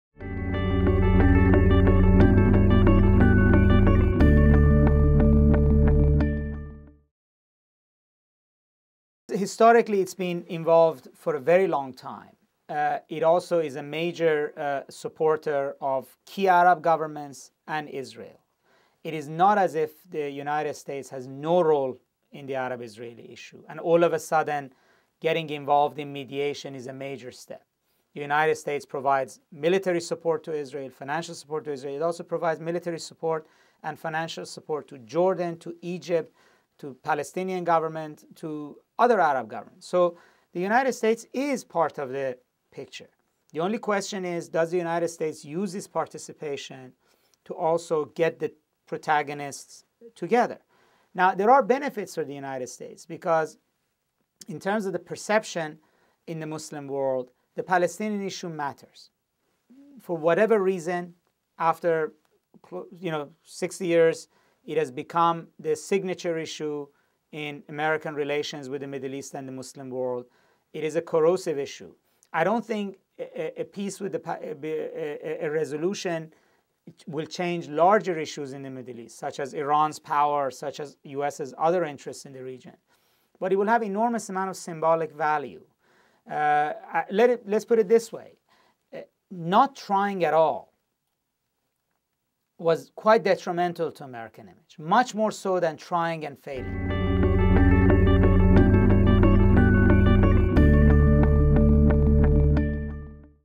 Vali Nasr: Should the U.S. be involved in the Arab-Israeli conflict? | Big Think
Vali Nasr: Should the U.S. be involved in the Arab-Israeli conflict?
Historically, the U.S. has been involved for a very long time.
Vali Nasr:

Vali Nasr is an Iranian-American political commentator and scholar of contemporary Islam. Born in Iran, Nasr and his family immigrated to the United States following the 1979 Islamic Revolution. Nasr received a BA from Tufts University in 1981 and a masters from the Fletcher School of Law and Diplomacy in 1984. He earned his PhD from MIT in political science in 1991.

Known for his view that wars within Islam will shape the future, Nasr has testified before Congress and has advised the President and Vice-President regarding sectarian violence in Iraq. Nasr is the author The Shia Revival, Democracy in Iran, and The Islamic Leviathan.

He has taught at the University of San Diego and the Naval Postgraduate School, and is currently a Senior Fellow at the Belfer Center at Harvard and Professor of International Politics at Tufts. A Life Member of the Council on Foreign Relations, Nasr has been published in Foreign Affairs, The New Republic, Foreign Policy, Time, the New York Times, and the Washington Post, among others. He is an editor of the Oxford Dictionary of Islam and has appeared on CNN, the BBC, National Public Radio, and not least of all The Daily Show with Jon Stewart and The Colbert Report.
TRANSCRIPT:

Question: Should the U.S. be involved in the Arab-Israeli conflict?

Vali Nasr: Well because historically it’s been involved for a very long time. It also is a major supporter of key Arab governments and Israel. It is not as if the United States has no role in the Arab-Israeli issue, and all of a sudden getting involved in mediation is a major step.

The United States provides military support to Israel, financial support to Israel. It also provides military support and financial support to Jordan, Egypt, Palestinian government, to other Arab governments. So the United States is part of the picture.

The only question is, does the United States use its participation to also get the protagonists together? Now there are benefits for the United States, because in terms of the perception in the Muslim world, the Palestinian issue matters. For whatever reason after 60 years, it has become the signature issue in American relations with the Middle East and the Muslim world. It is a corrosive issue.

I don’t think peace with a resolution will change larger issues in the Middle East, such as Iran’s power, such as U.S.’ other interests in the region. But it will have an enormous amount of symbolic value.

Let’s put it this way. Not trying at all was quite detrimental to American image, much more so than trying and failing.